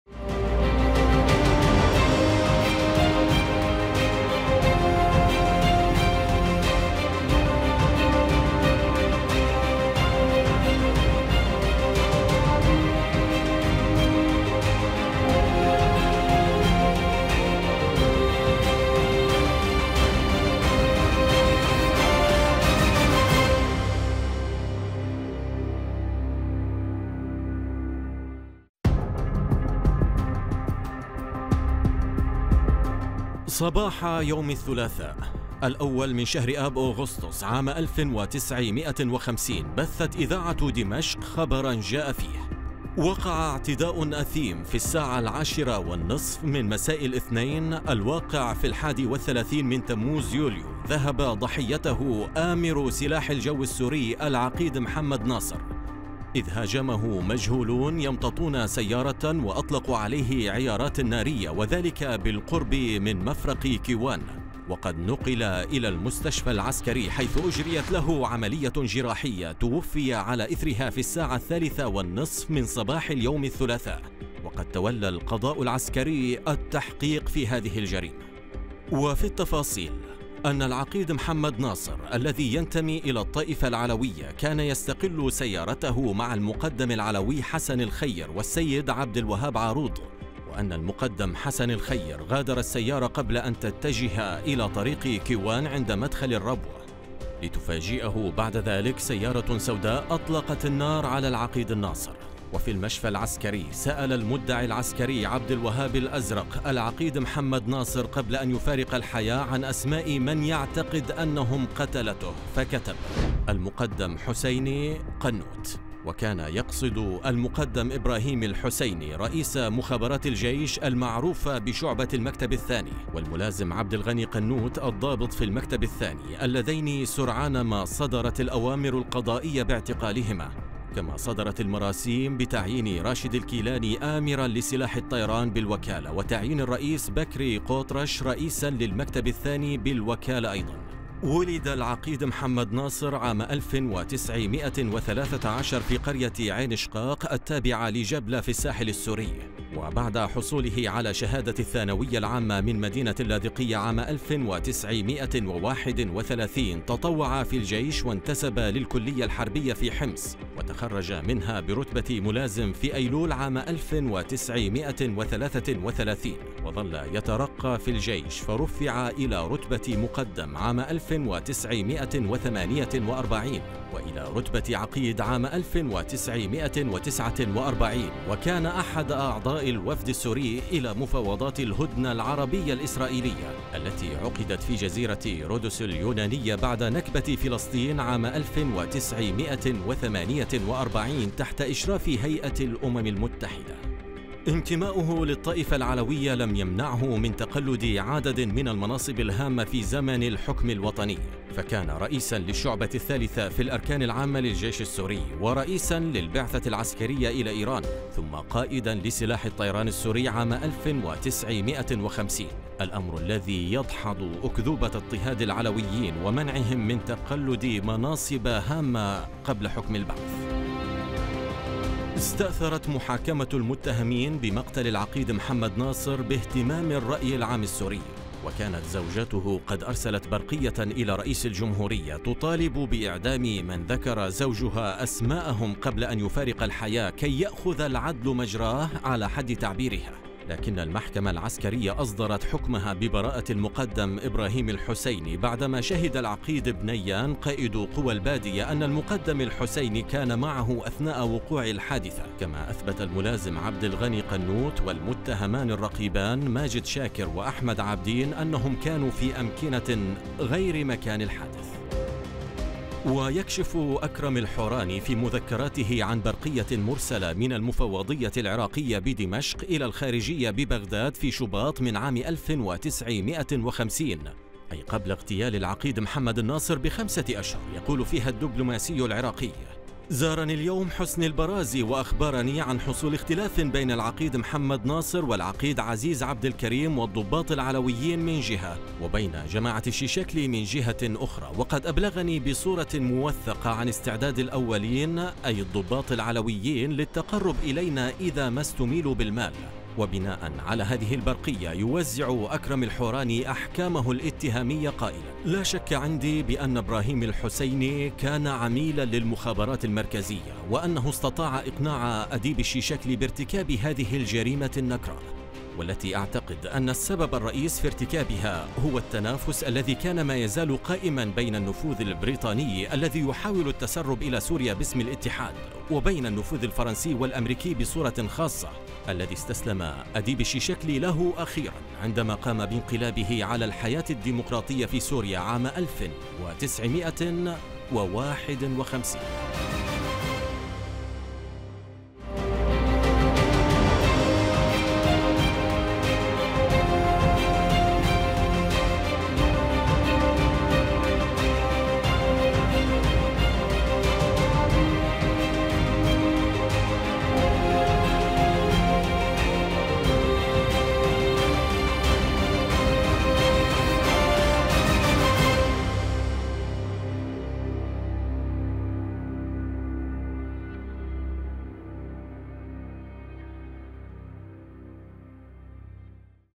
اغتيال قائد سلاح الطيران السوري | موسوعة سورية السياسية
موسوعة مصورة ترصد تاريخ سورية السياسي المعاصر برؤية بصرية وتوثيقية تسعى لتقديم الحدث بكل أبعاده وخلفياته. في الحلقة الثانية والتسعون: (اغتيال قائد سلاح الطيران السوري)  نتوقف عند حدث اغتيال الضابط العلوي الذي كان يشغل منصب قائد سلاح الطيران في الجيش السوري عام 1950 لنكتشف أن العلويين كانوا حاضرين في الجيش وفي المناصب الحكومية منذ عهد الاستقلال، وأن أكذوبة اضطهادهم ومنعهم من تسلم المناصب الحكومية، كانت حجة لاستيلائهم على الحكم بعد انقلاب الثامن من آذار عام 1963 الذين أوصلهم شيئا فشيئا إلى السيطرة على الجيش والمخابرات.
نص وإخراج: محمد منصور
قراءة التعليق: سليمان عبد المولى
مونتاج: البرّاق سليمان